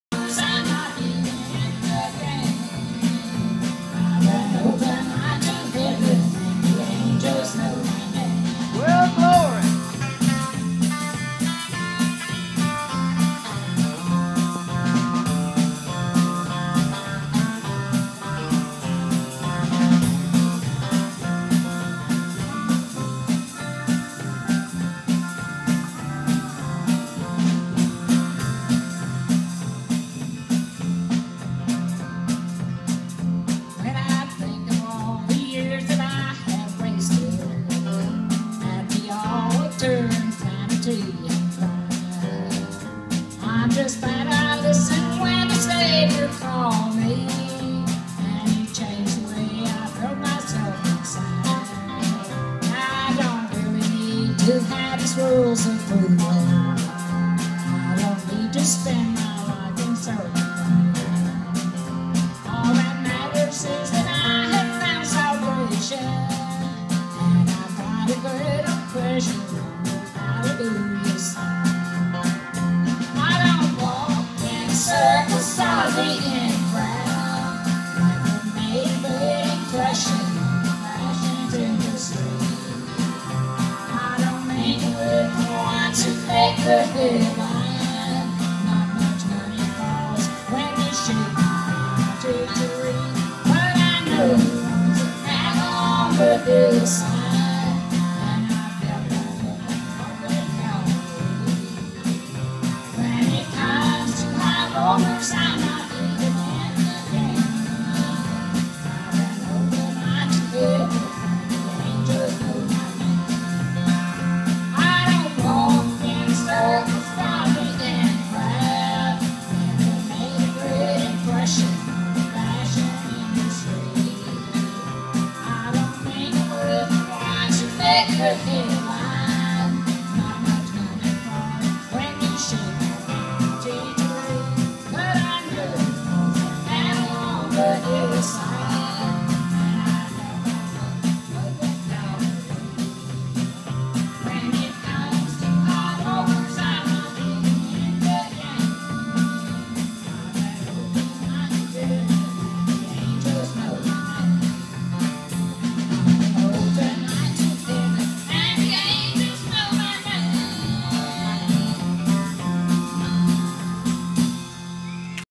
Grandma singing
My Grandma singing at Eagles Nest Church